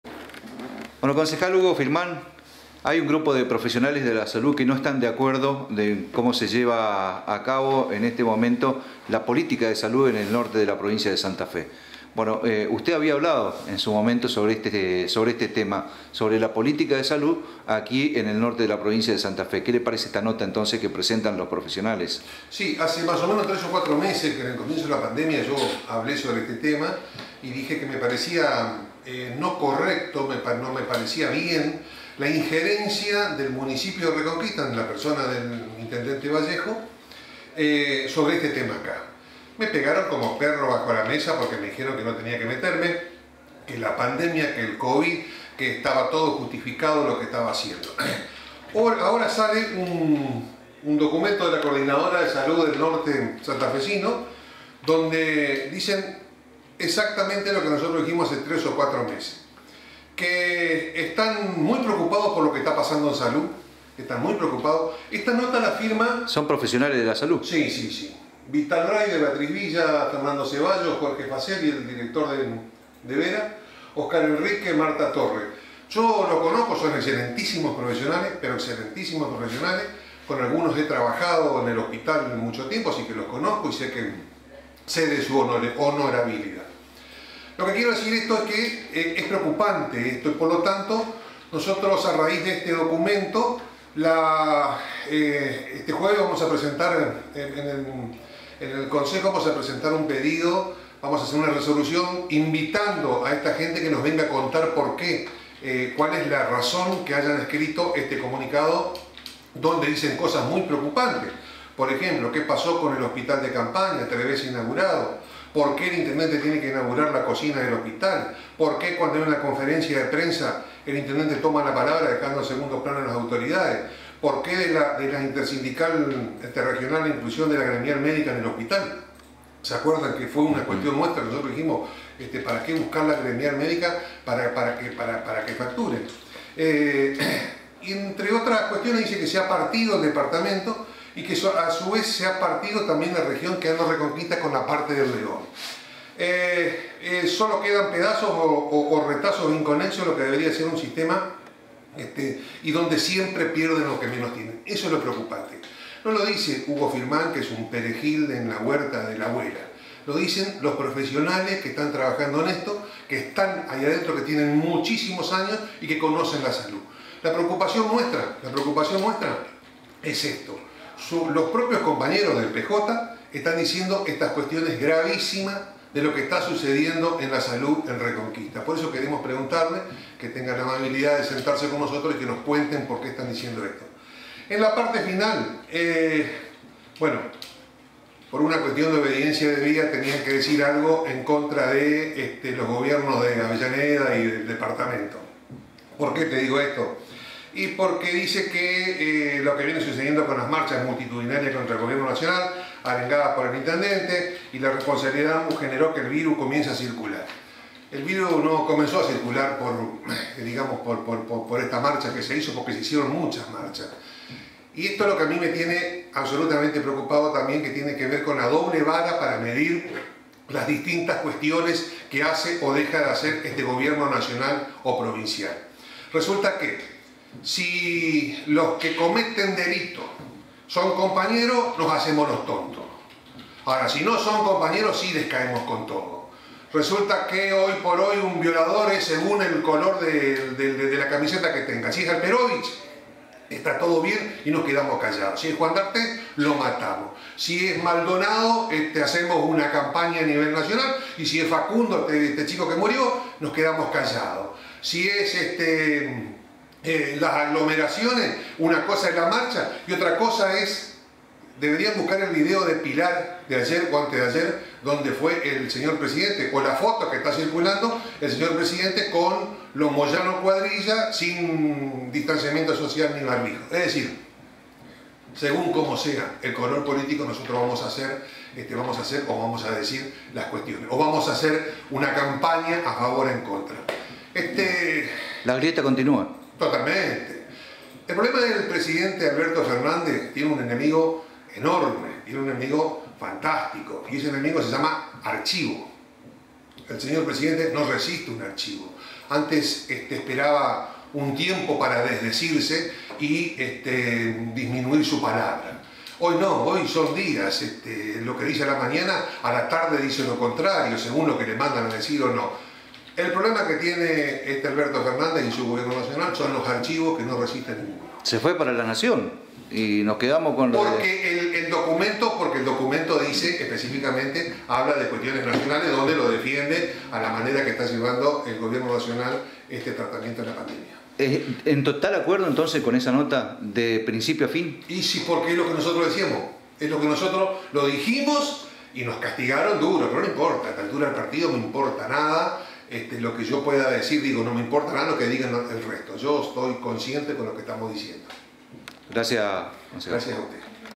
Firman habla de Salud, politica y Justicia
El Concejal Hugo Firman dio una nota para la camara de Pedro Flores, de la DMH.
Contenido editado y subido por
          EDGARDO QUARIN